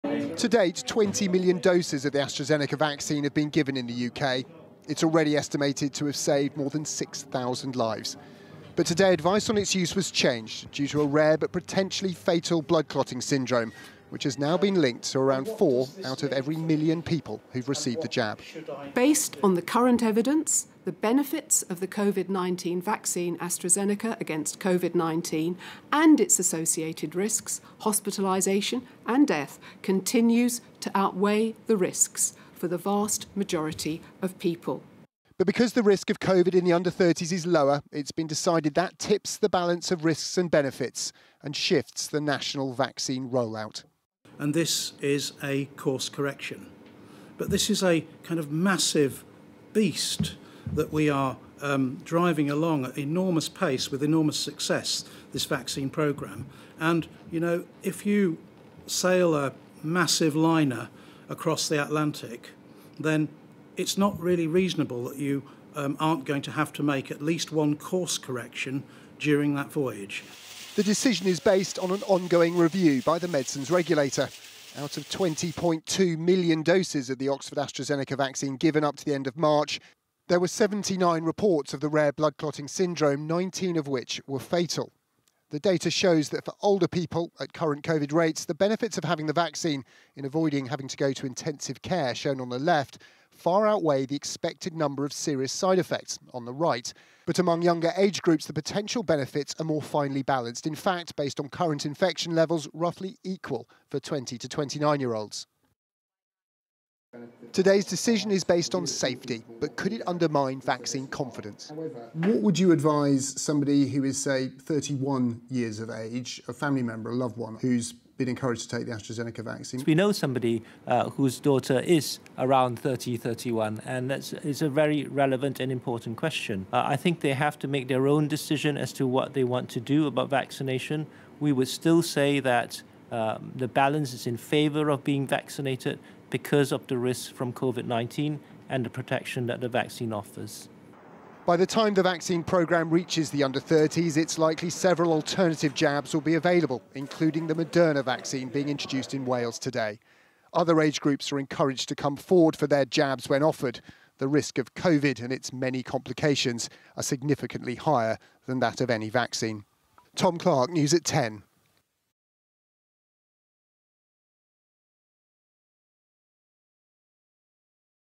Under 30s in UK won't be given AstraZeneca vaccine over 'extremely rare' blood clot risk | ITV News
The Medicines and Healthcare products Regulatory Agency (MHRA) in the UK said there were still huge benefits of the vaccine in preventing Covid-19, and has not concluded that the vaccine causes rare clots, although it says the link is getting firmer.